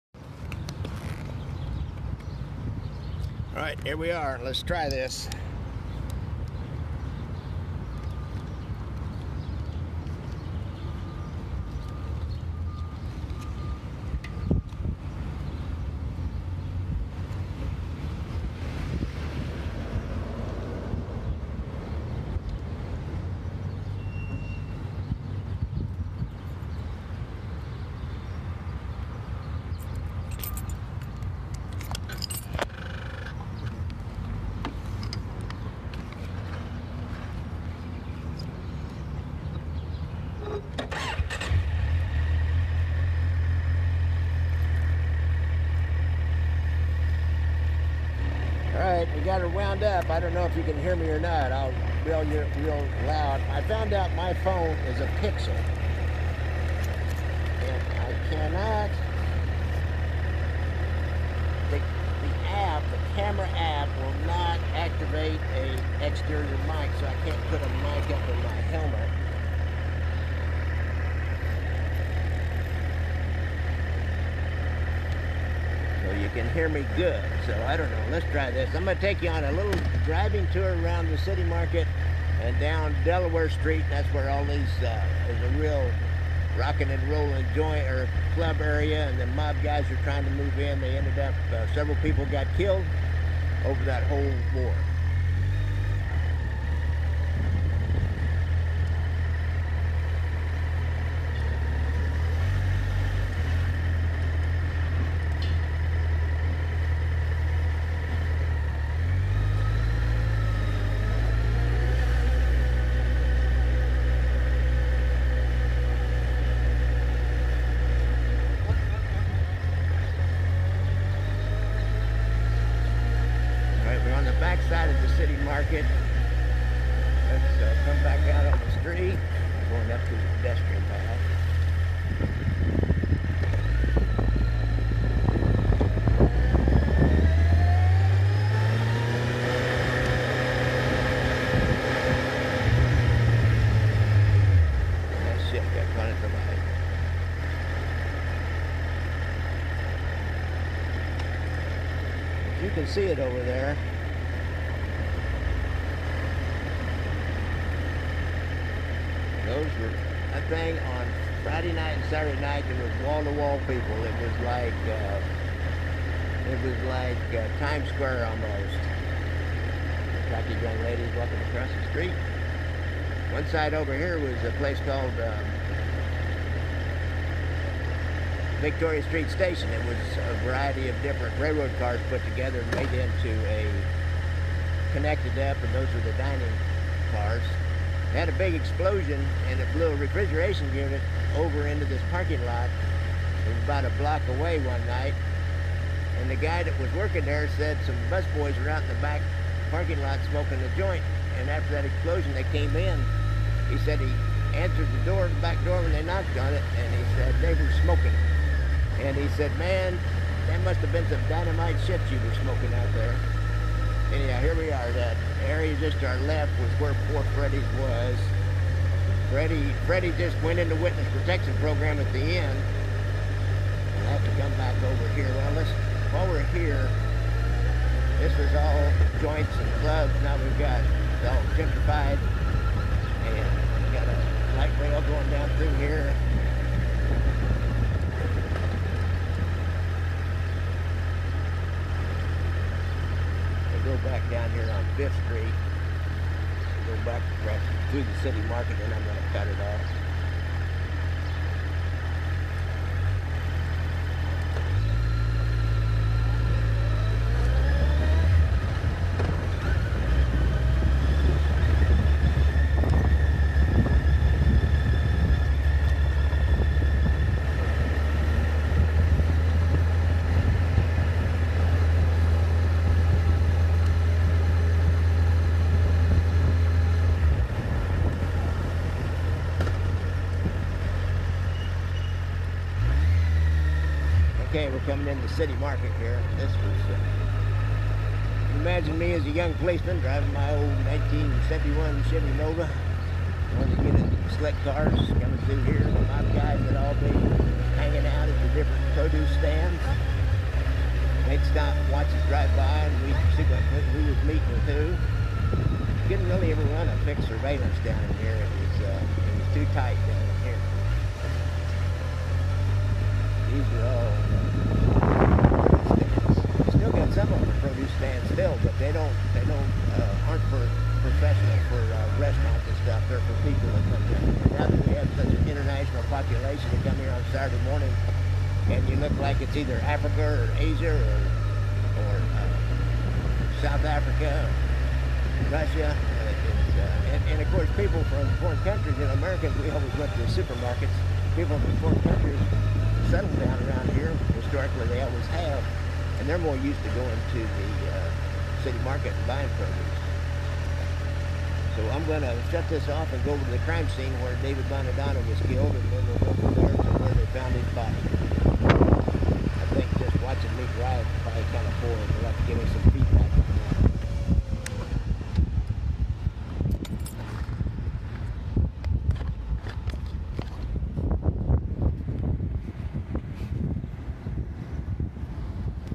Facebook Live Mob Tour Part 3 River Quay and City market
We go to the old city market area where the KC mob started back in the Black Hand days and continued into the 1990s. We will go out and around to the River Quay where a mob war started over parking lot leases.